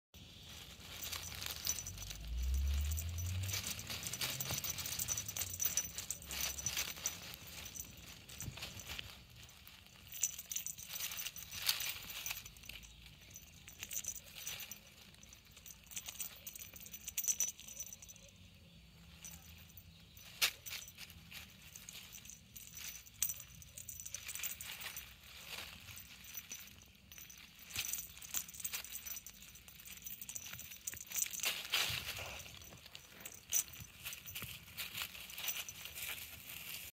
GR*Elvenstar's Titan 11 weeks old
Our blue Norwegian Forest prince had his first walk in our olive grove!